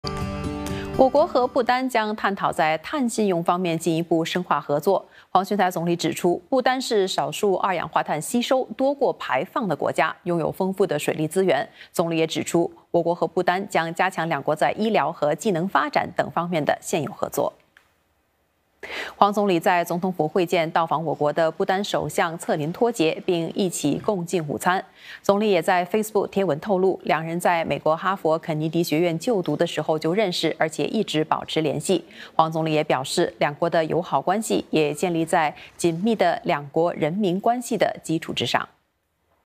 我国和不丹将探讨在碳信用方面进一步深化合作
我国和不丹将探讨在碳信用方面进一步深化合作。黄循财总理指出，不丹是少数二氧化碳吸收多过于排放的国家，拥有丰富的水力资源。总理也指出我国和不丹也将加强两国在医疗和技能发展等方面的现有合作。